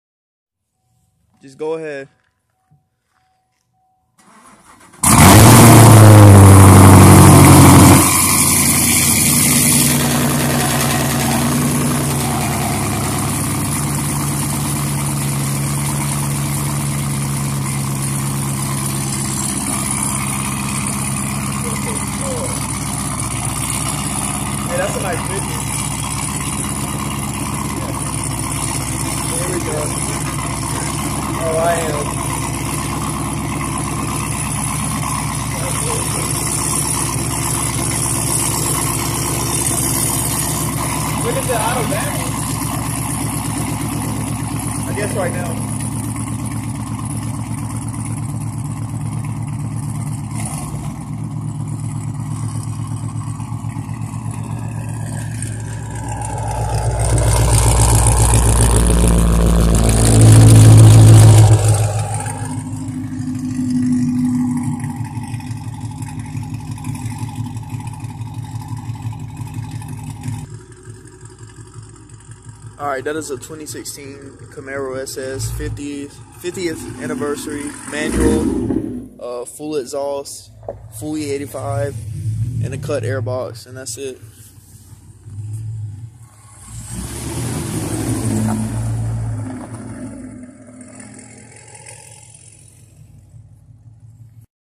2017 Camaro SS Brutal Cold Start
Sorry I said the Camaro was a 2016 its actually a 2017 lol. Camaro also has 2 step colder spark plugs.